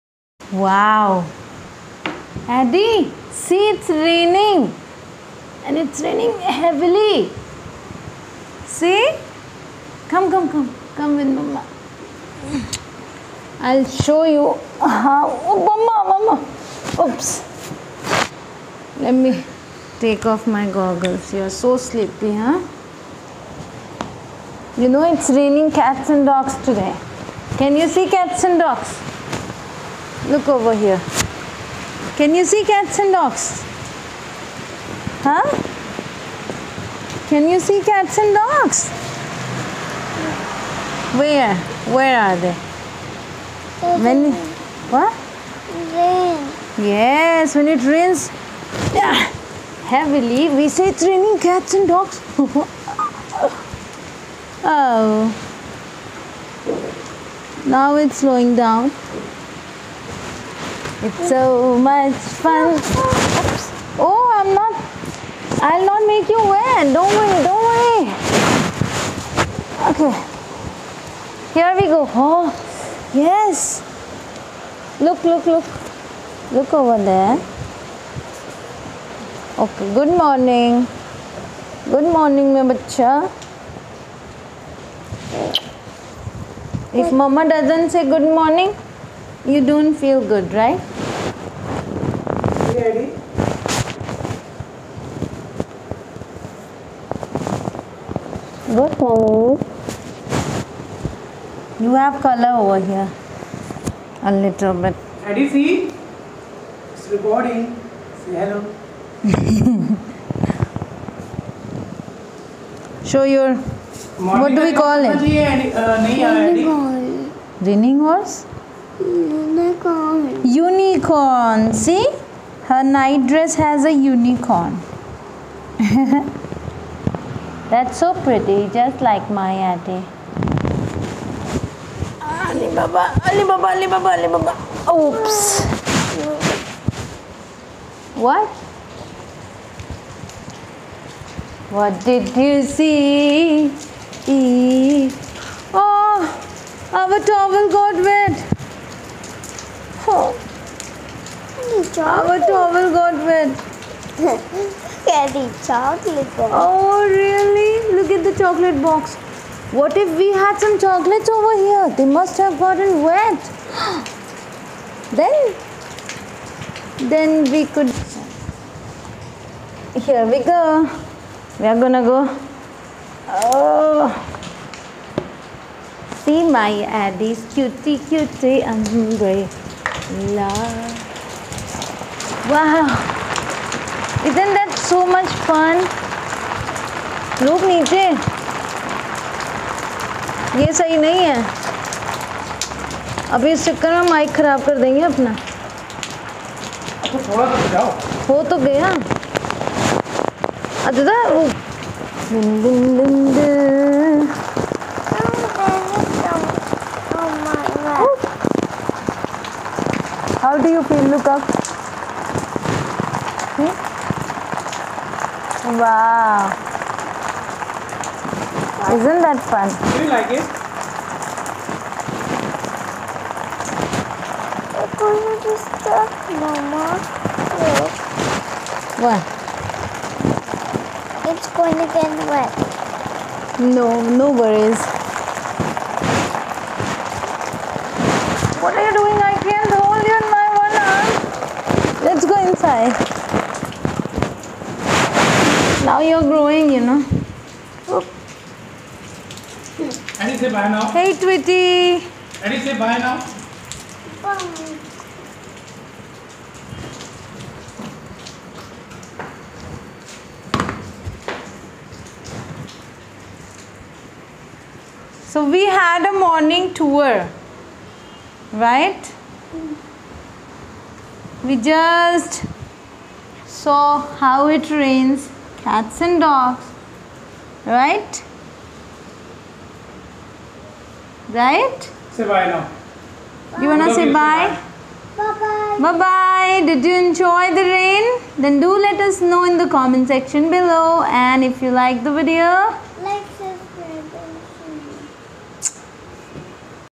Pichhali Baarish Ki Yaaden | RKfam
Guys, aaj hum aapse pichhali baarish ki yaaden share kar rahe hain, I hope you will enjoy this Family Vlog.

👉 Learn Adi n Daddy English Shorts

YouTube Channels:@EnglishConnectionByKanchan  @VidyaConnection